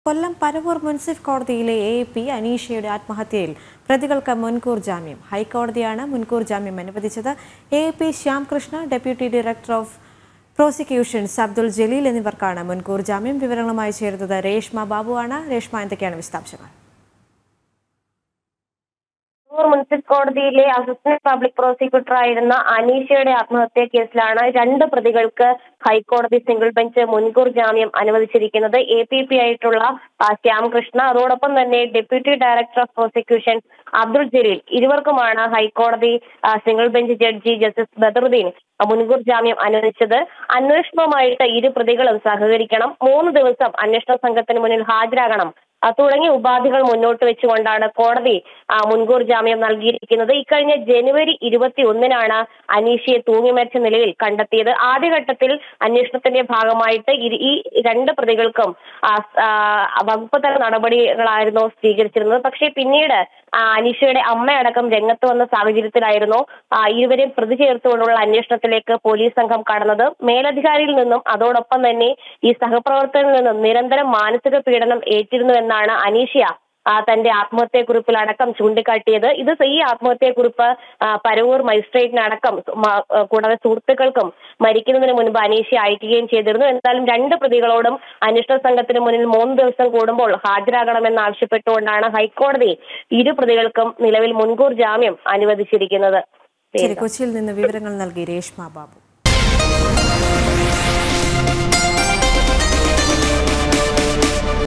അനീഷയുടെ ആത്മഹത്യയിൽ പ്രതികൾക്ക് മുൻകൂർ ജാമ്യം
കൊല്ലം പരവൂർ മുൻസിഫ് കോടതിയിലെ എ.പി.പി അനീഷയുടെ ആത്മഹത്യയിൽ പ്രതികൾക്ക് മുൻകൂർ ജാമ്യം. ഹൈക്കോടതിയാണ് മുൻകൂർ ജാമ്യം അനുവദിച്ചത്. എ.പി.പി ശ്യാം കൃഷ്ണ, ഡെപ്യൂട്ടി ഡയറക്ടർ ഓഫ് പ്രോസിക്യൂഷൻസ് അബ്ദുൽ ജലീൽ എന്നിവർക്കാണ് മുൻകൂർ ജാമ്യം.